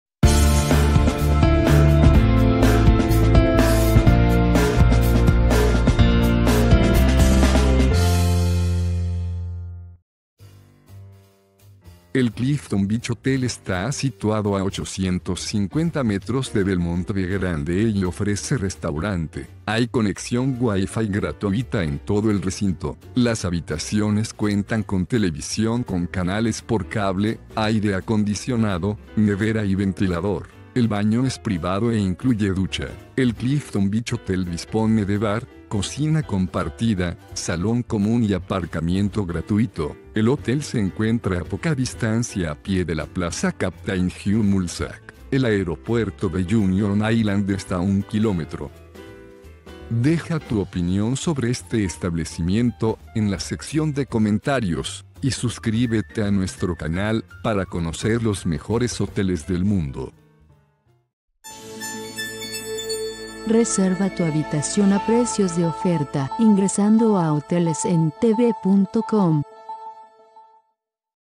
Clifton Beach Hotel, San Vicente y las Granadinas
Clifton Beach Hotel es un Hotel con restaurante, servicio de habitaciones y salas de reuniones / banquetes de Clifton ubicado en Union Island. Su amable personal te asistirá en cualquier problema que puedas tener durante tu estancia.